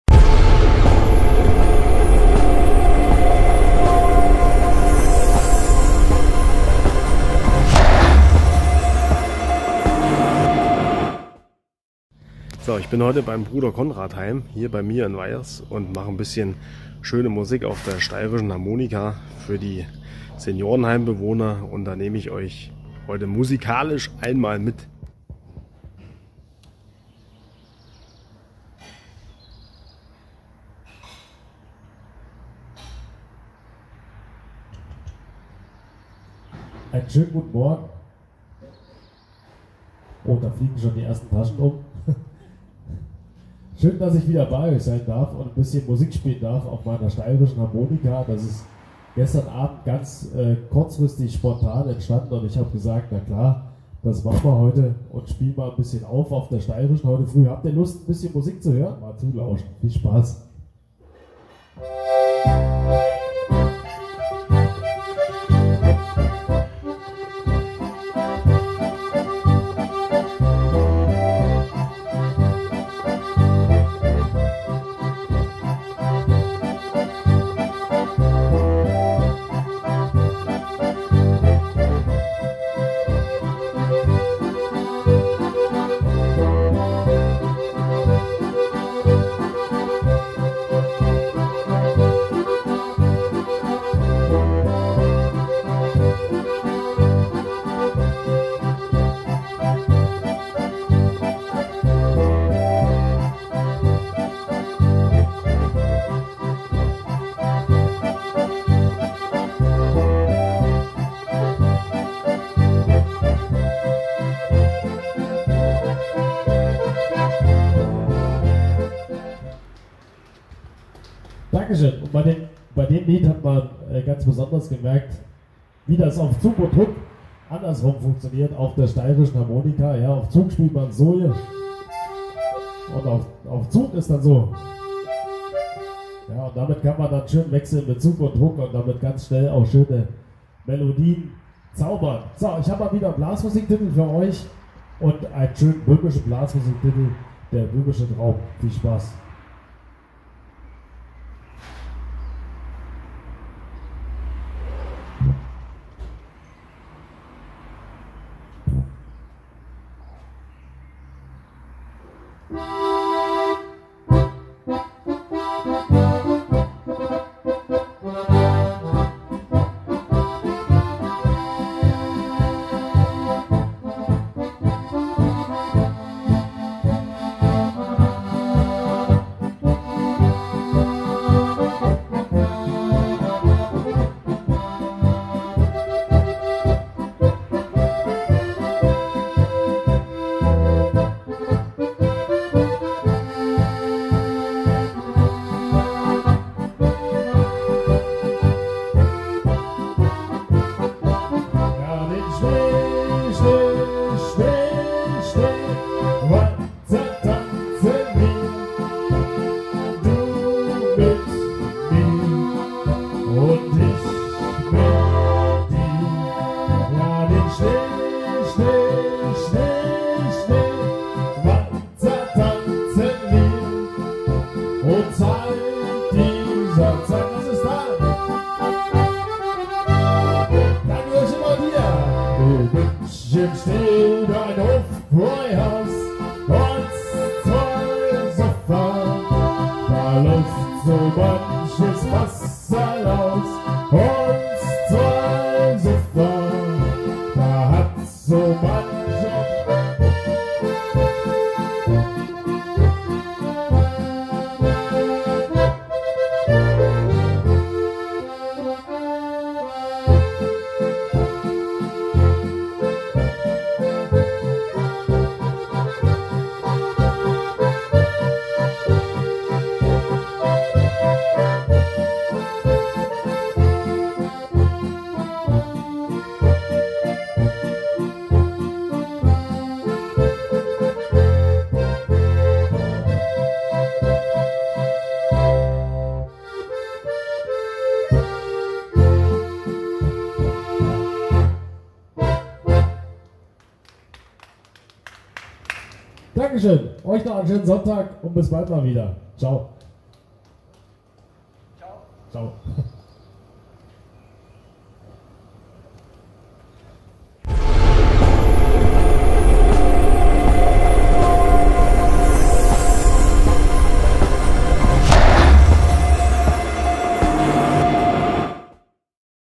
Wir tun etwas gutes für die Senioren in der Rhön
Gerade in der Corona Zeit ist es für die Bewohner von Seniorenheimen sehr schwer. Kein Besuch, ein hohes Risiko einer Infektion und damit auch weniger Lebensmut 🙄 Da wollten wir von Rhön Harmonika helfen und haben ein spontanes Konzert in Weyhers gegeben 😍